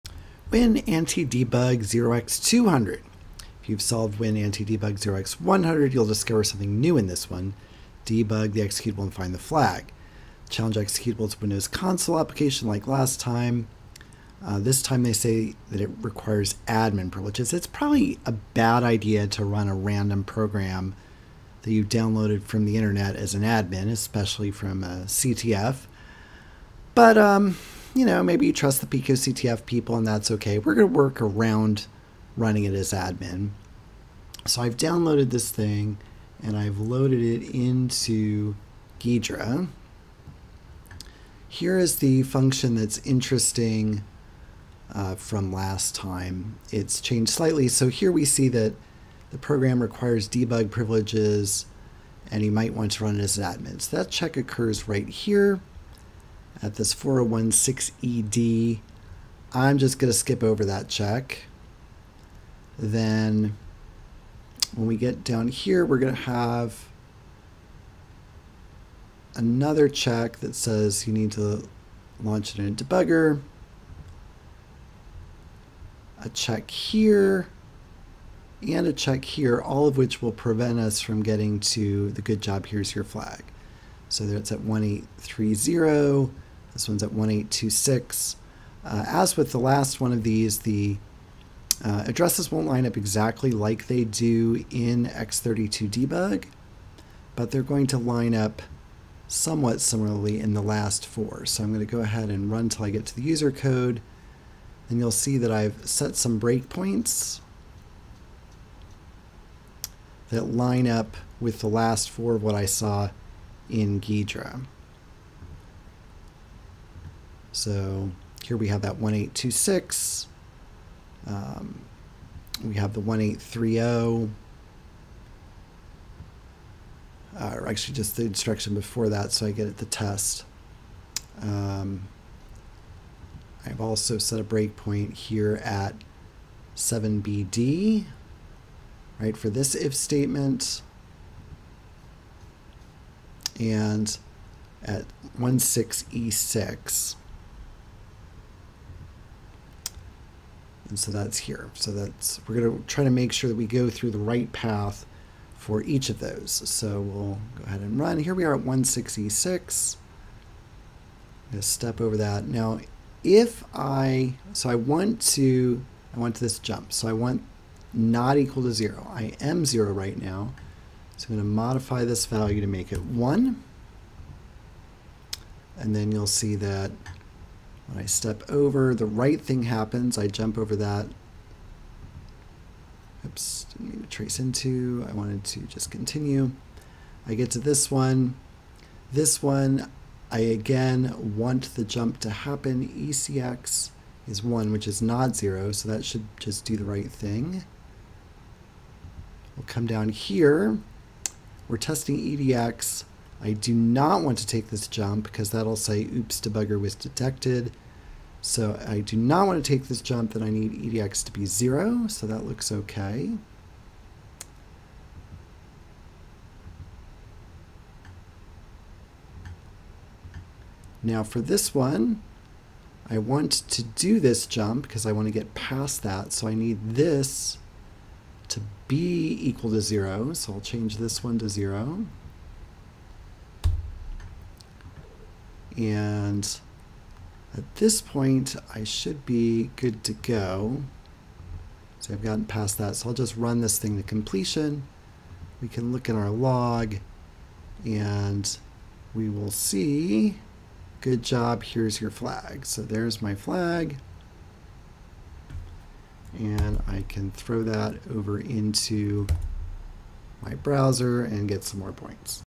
pico2024 WinAntiDbg0x200
More use of x32dbg to set registers to get to the part of the code where it prints out the flag. Avoided having to run this one as administrator (though probably you should be running these in a virtual machine).